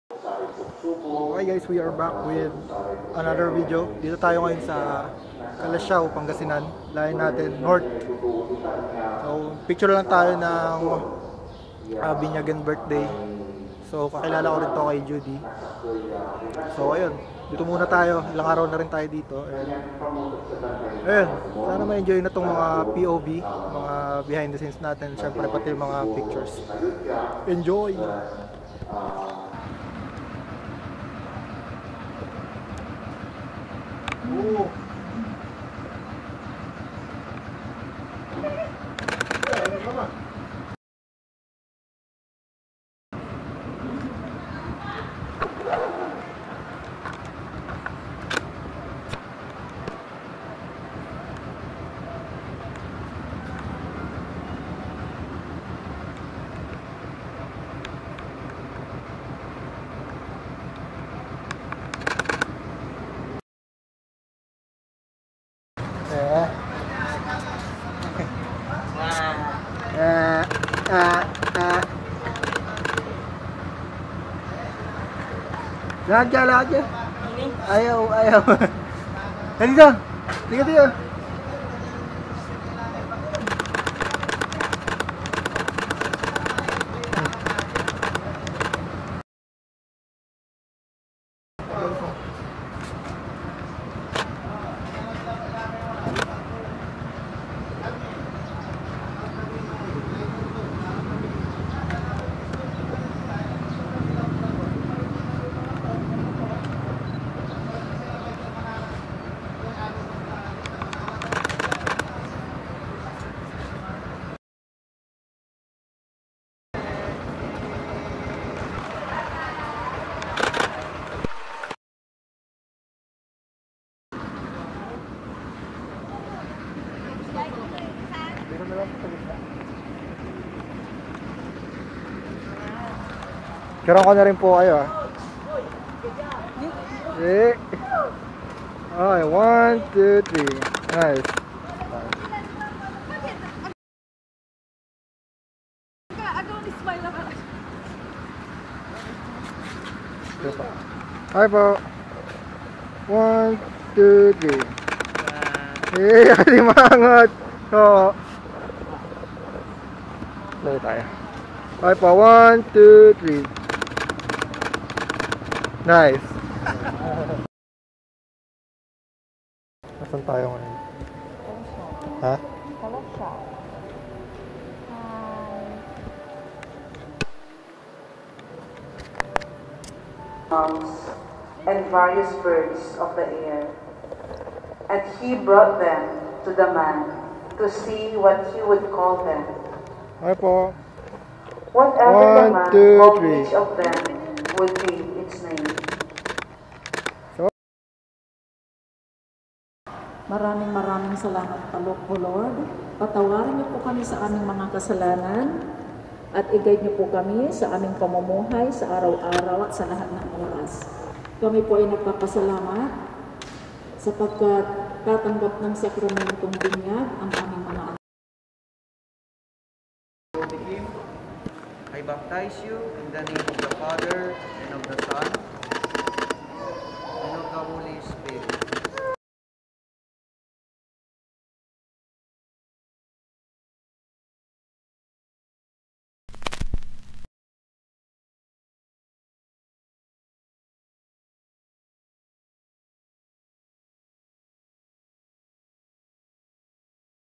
Christening Shoot | POV | Photo Coverage in Pangasinan
Dito tayo sa Pangasinan, konting POV photos lang about sa Christening na nacover natin. Naubusan agad battery action cam natin. Ito yung ilang photos na nakuha natin don. Salamat sa panonood, takits tayo sa susunod na video.

LET'S TALK!